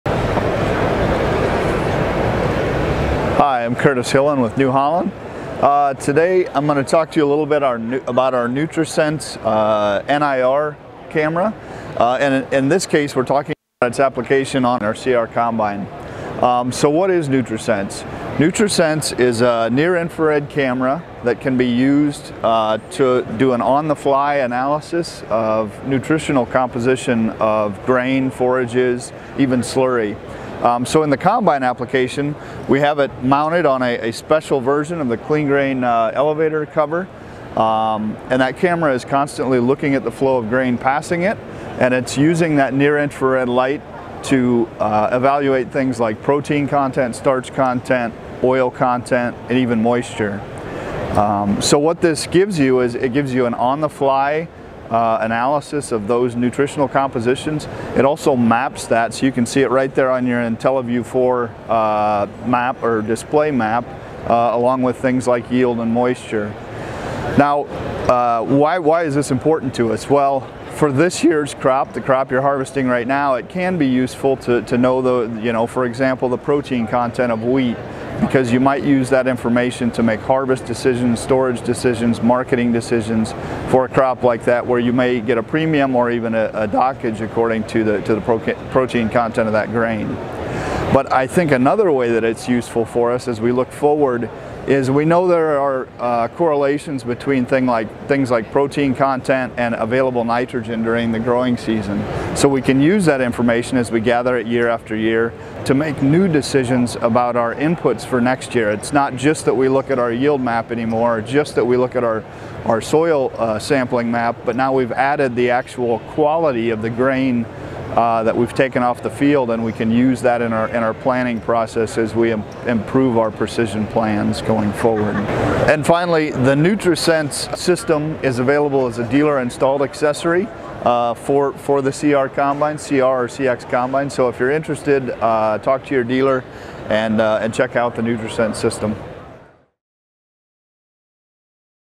NutriSense™ Technology on New Holland Combines | Commodity Classic 2023
Curtis Hillen talks about NurtiSense™, which utilizes a Near Infrared (NIR) sensor to analyze, record, and map crop composition in real-time during harvest.